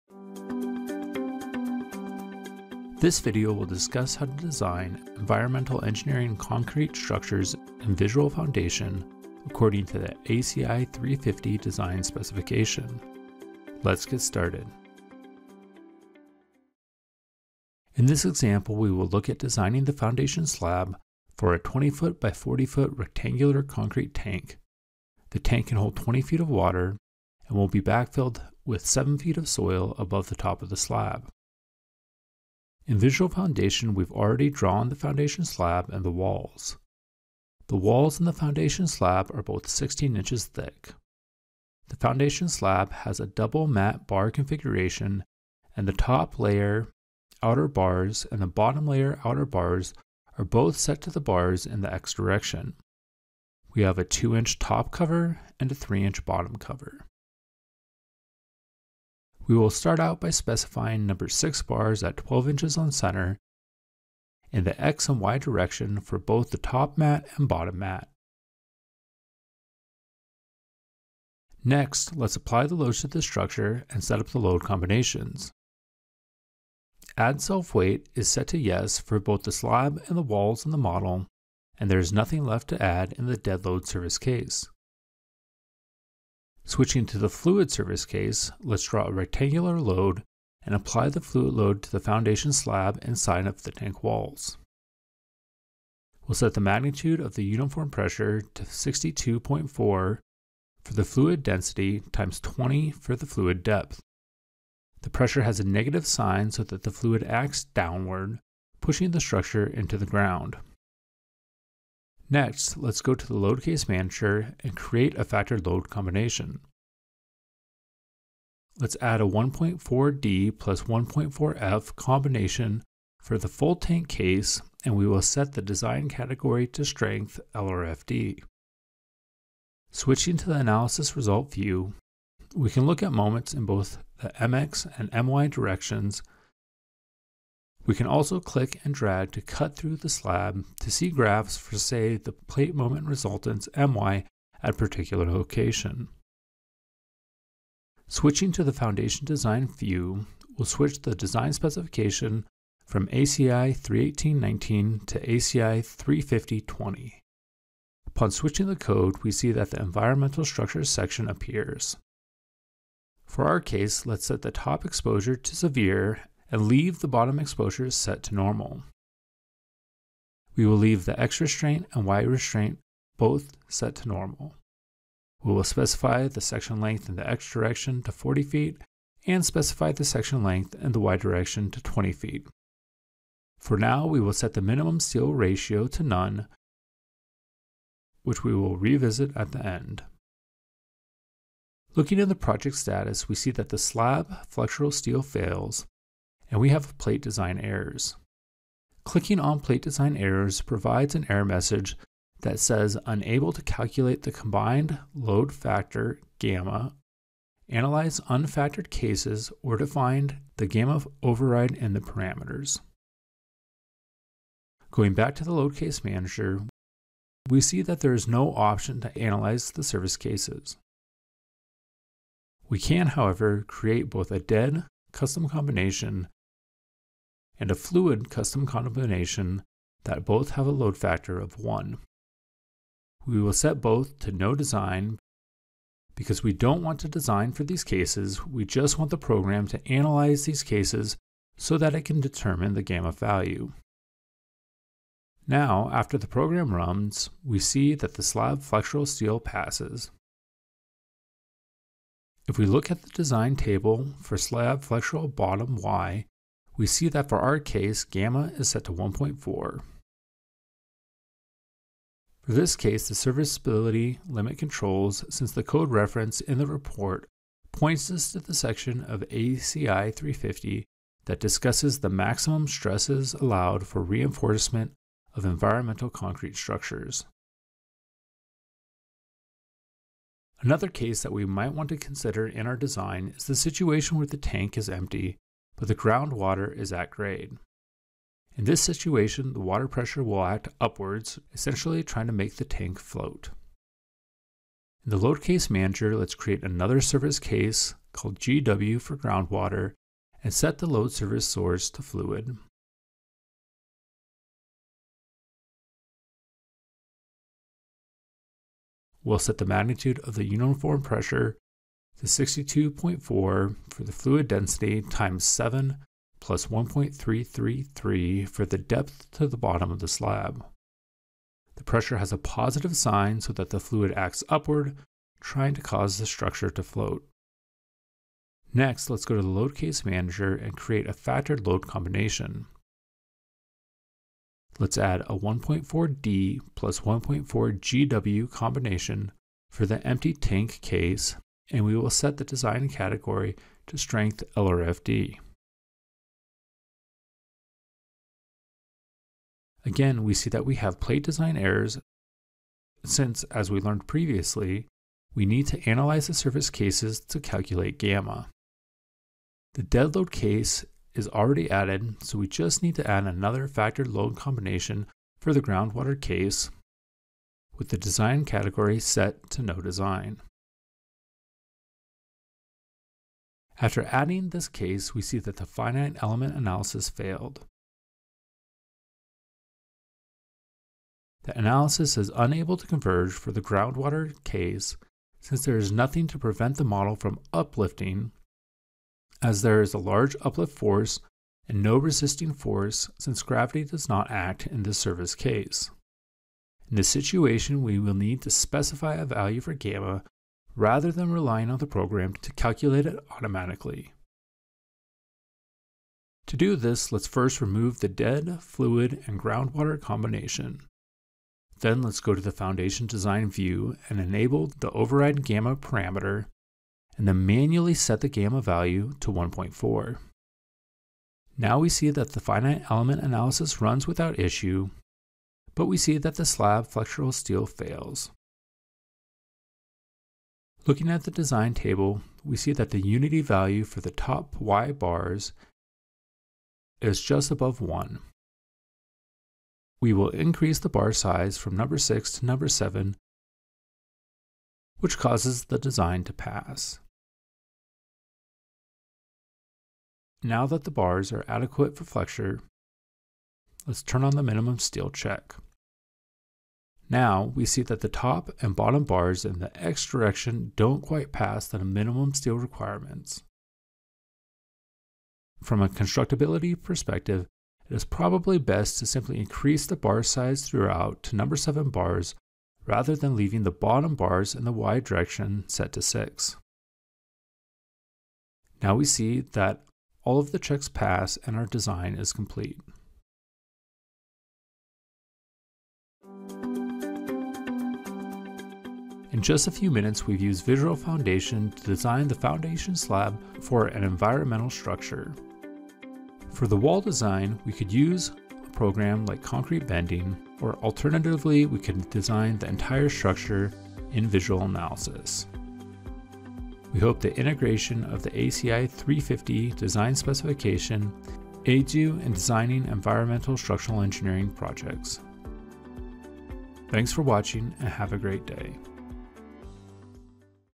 VF: ACI 350 Design
This video provides an overview of how to design environmental concrete structures according to the ACI 350 design specification in VisualFoundation.